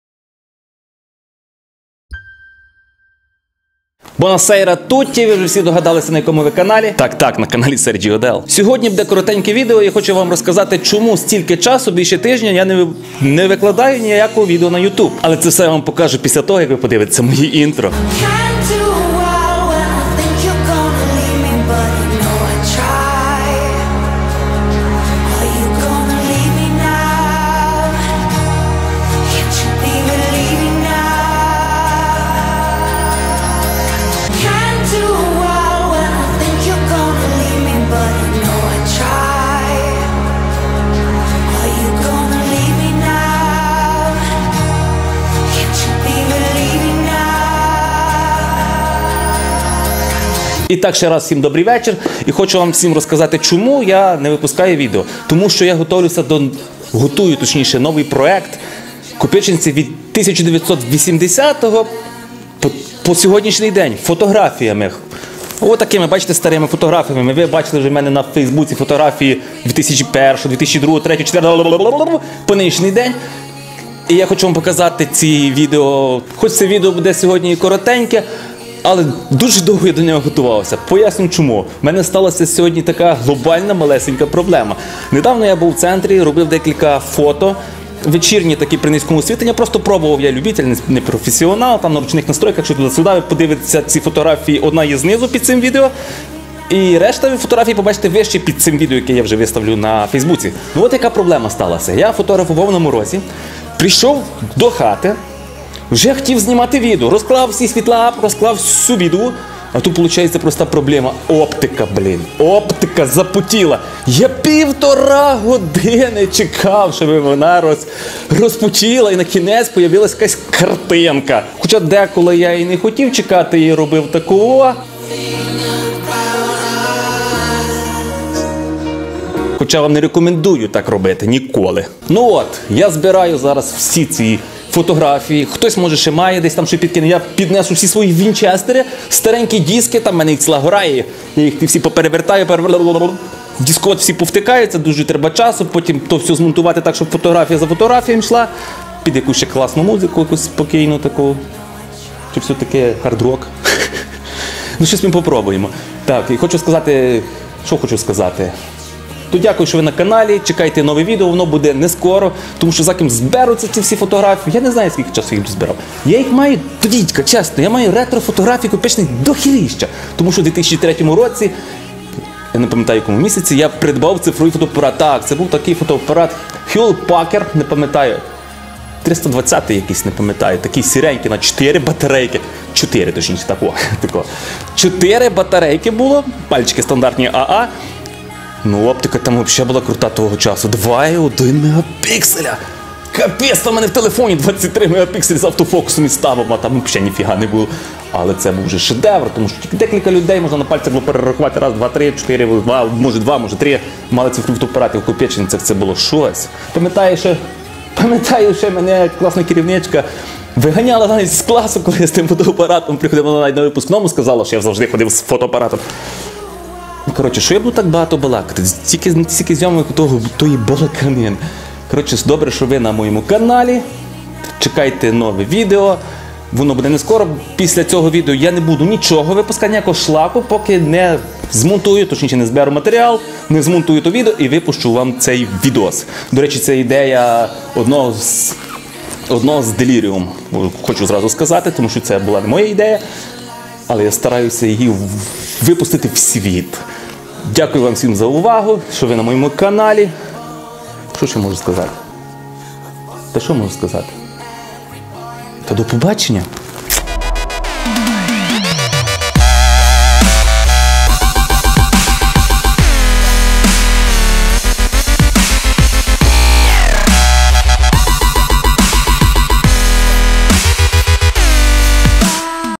ЗВЕРНЕННЯ SERGIO DELL! НОВИЙ ПРОЕКТ. СПОГАД ПРО КОПИЧИНЦІ! 1980-2019рр.
Я SERGIO DELL. ХОЧУ І МАЮ МОЖЛИВІСТЬ СТВОРИТИ СУЧАСНИЙ ПРОЕКТ ПРО ІСТОРІЮ КОПИЧИНЕЦЬ У ФОТО ЗА ДЕКІЛЬКА ДЕСЯТИЛІТЬ! САМОМУ БУДЕ МЕНІ ВАЖКО І ДОВГО, ПРОШУ ПІДТРИМАЙТЕ МЕНЕ! Я НЕ ФІНАНСОВО ПРОШУ, БО Я ІНТУЗІАСТ, А МОЖЕ ХТОСЬ МАЄ СТАРІ ФОТО, НАВІТЬ НА ПАПЕРІ, Я ЇХ ОЦИФРУЮ І ВКАЖУ АВТОРА!!!
МОНТАЖ: SERGIO DELL RECORDS.
ЗЙОМКА: SERGIO DELL RECORDS.